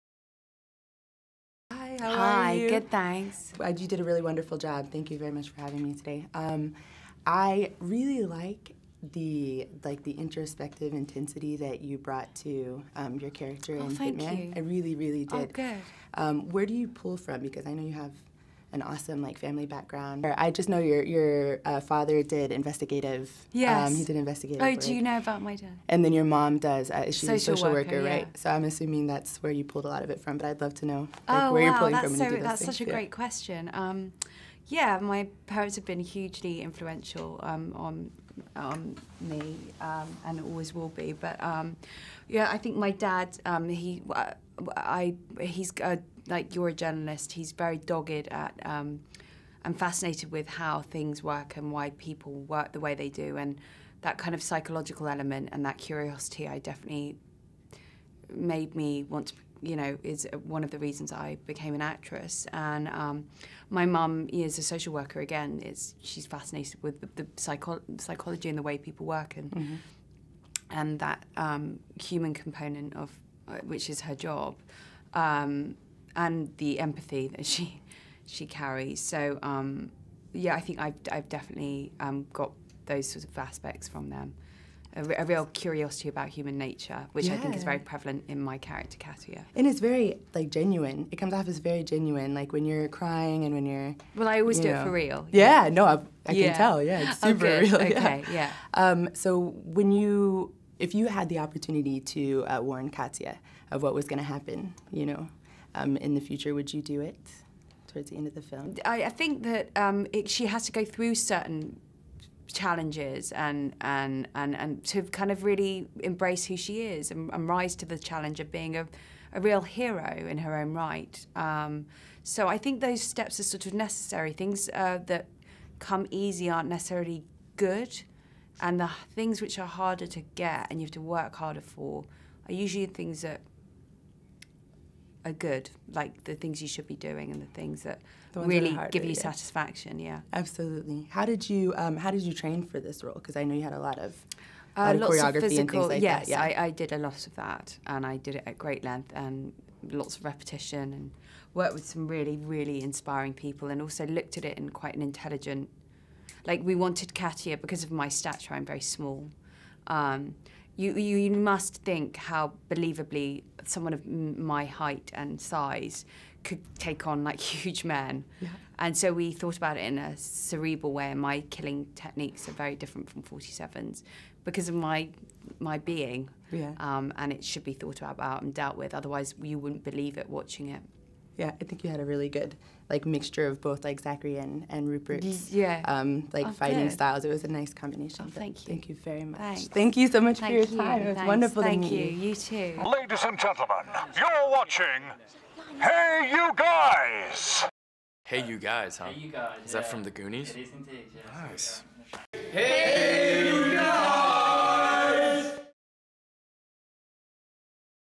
Hannah Ware Exclusive Interview - Hitman: Agent 47
Micah Phillips from HeyUGuys interviews Hannah Ware for her movie Hitman: Agent 47 at the junket in New York.

Hitman: Agent 47 is a movie spin-off from the computer / video game and stars Zachary Quinto, Rupert Friend, Hannah Ware, Emilio Rivera, Rolf Kanies, Ciarán Hinds and Thomas Kretschmann.